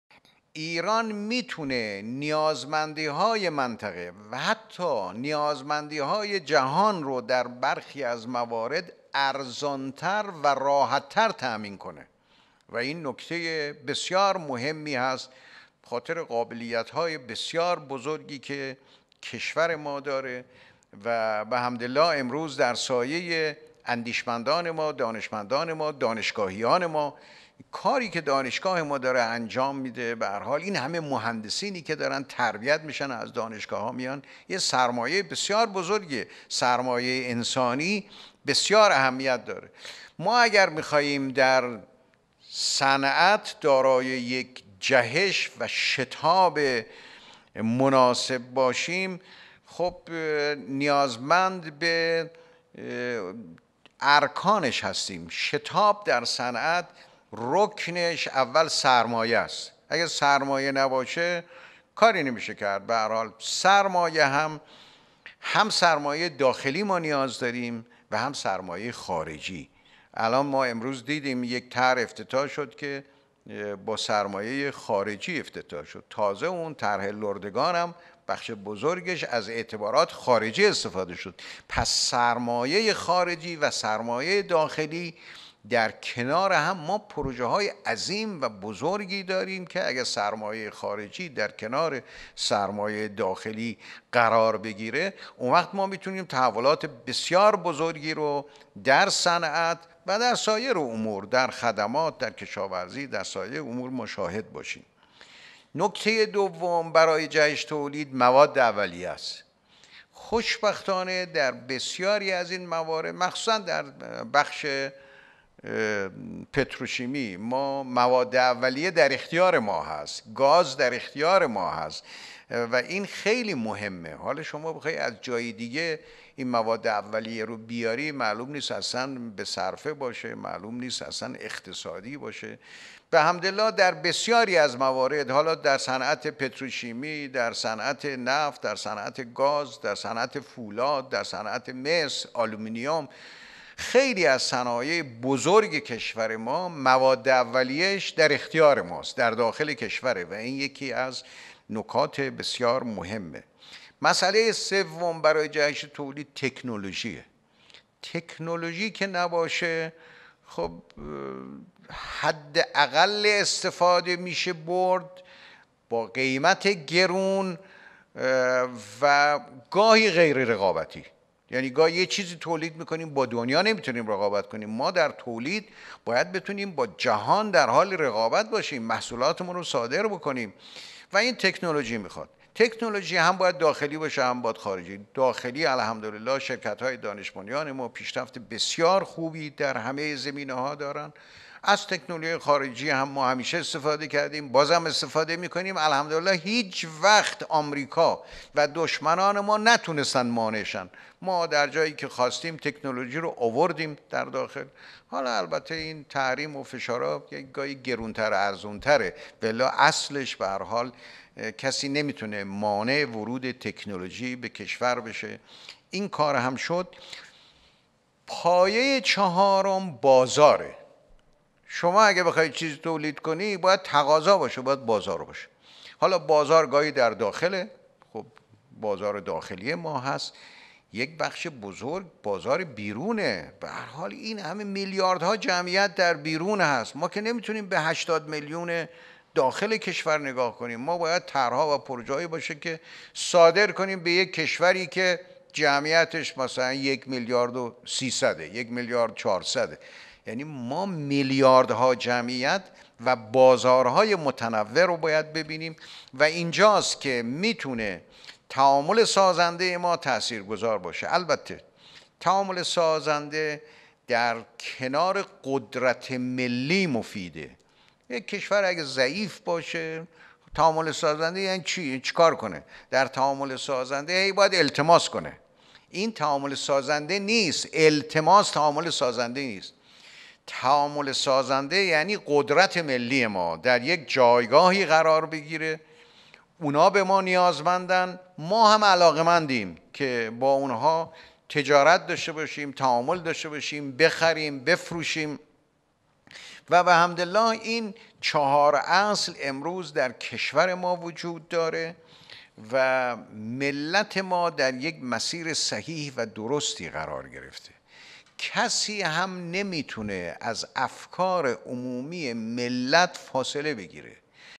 تعامل سازنده با جهان در کنار قدرت ملی مفید است
تبیین 4 اصل مهم تحقق جهش تولید در ایران اسلامی/ ایران می‌تواند نیازمندی‌های منطقه و حتی جهان را در برخی از موارد ارزان‌تر و راحت‌تر تأمین کند/تعامل سازنده با جهان در کنار قدرت ملی مفید است